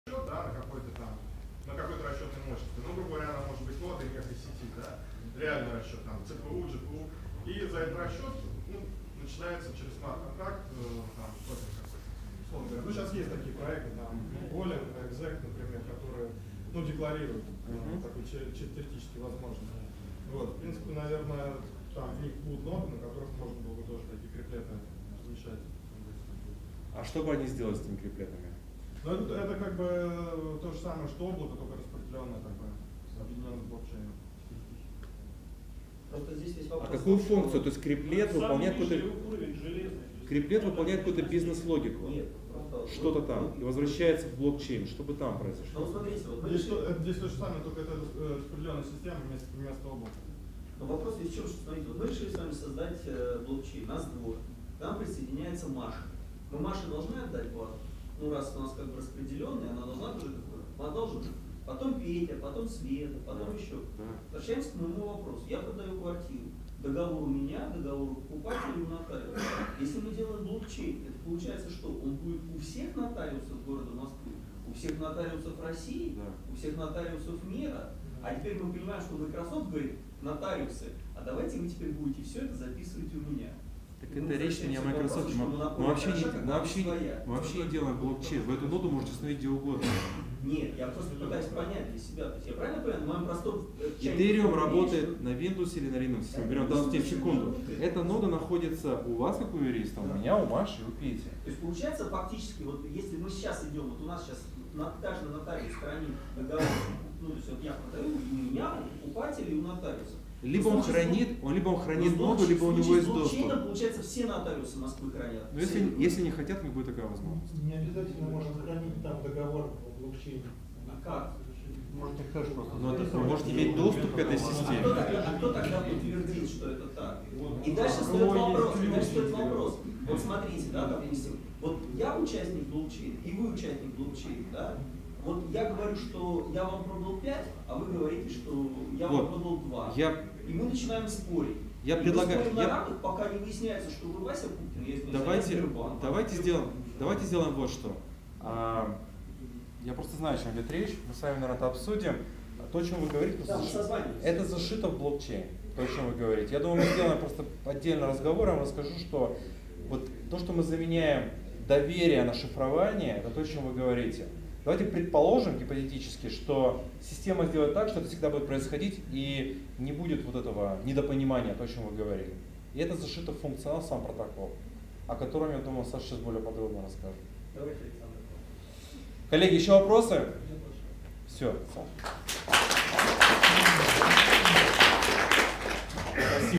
Криплеты - следующее поколение Оракулов - Kostya Goldstein ч.4
Moscow Blockchain Developers Meetup 3
Kostya Goldstein Технический евангелист Microsoft Russia выступит с докладом: "Криплеты - следующее поколение Оракулов":
- Проект Bletchley — реализация архитектурного подхода Microsoft к созданию экосистемы Enterprise Consortium Blockchain
- Криплеты — основные строительные блоки, позволяющие создать промежуточный слой в архитектуре с защитой Blockchain
- Инструменты для разработчиков, которые предоставляет Azure Blockchain as a Service